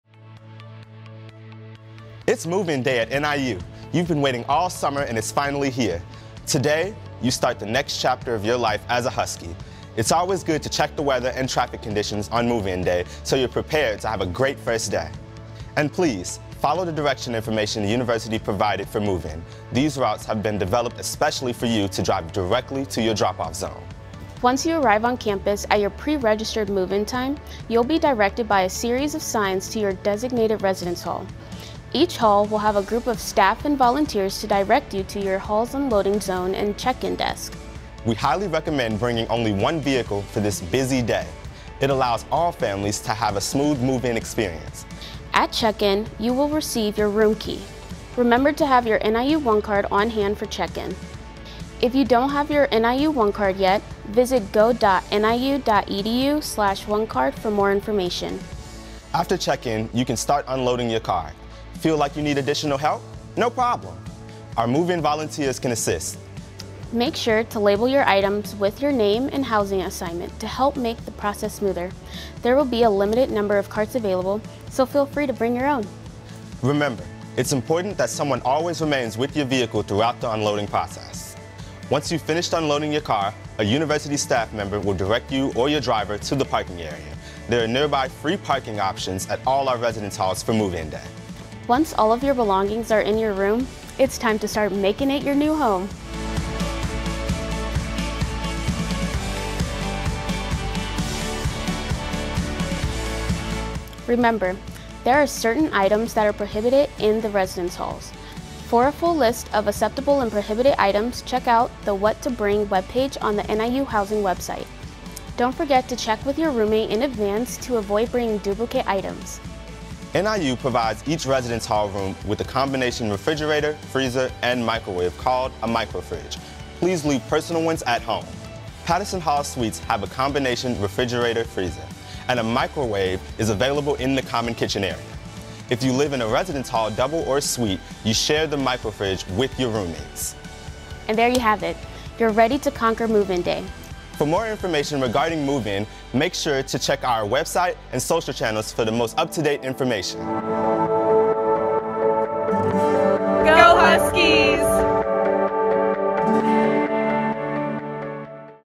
It's Move-in Day at NIU!
Welcome to Move-in day at NIU. Start making your room into your new home! Resources mentioned in this video include:

    • What to Bring webpage: What to Bring - NIU - Housing and Residential Services
        go.niu.edu/bring
    • Moving In webpage: Move-in Day Guide and What to Bring - NIU - Housing and Residential Services
        go.niu.edu/movein
    • Onecard: OneCard ID Services | Northern Illinois University (niu.edu)
        go.niu.edu/onecard
    • Facebook and Instagram handle: @niuhousing
        niu.edu/housing